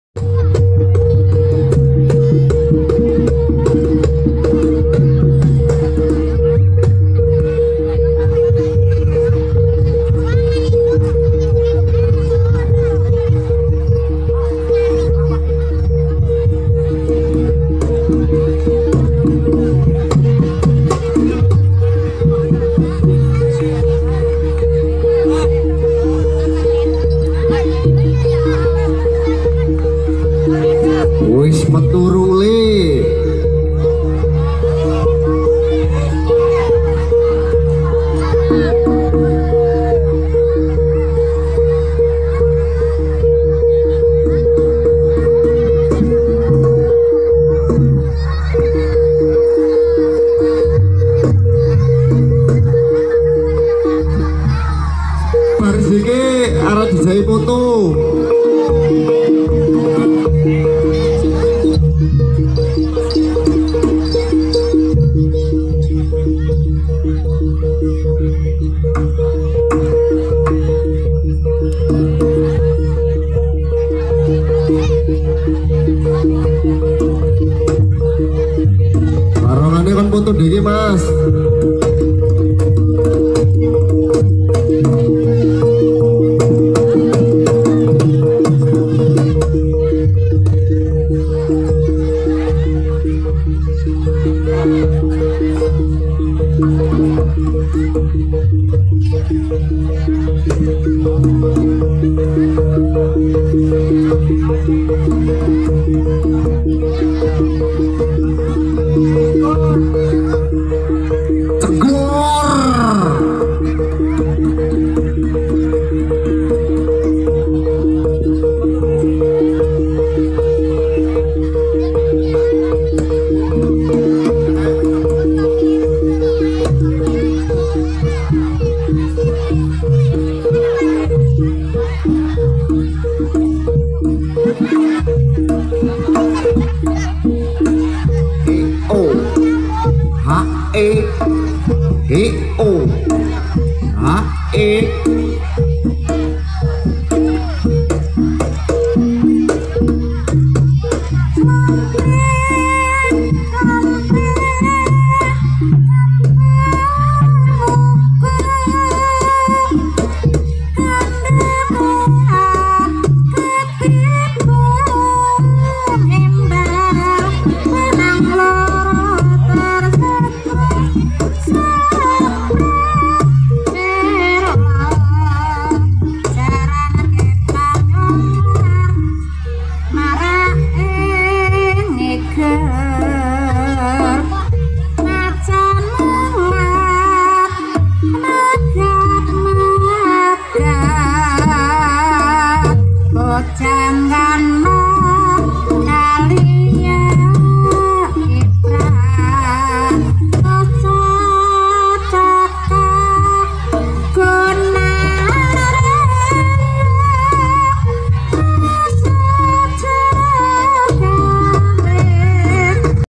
Jaran Abang Ndadi..‼️kuda kepang Sekar muda pajaresuk, live Pardasuka, Pringsewu, Lampung.
Jaran Abang Ndadi..‼️ seni tari kuda kepang Sekar muda pajaresuk Pringsewu, live Pardasuka, Pringsewu, Lampung, Indonesia.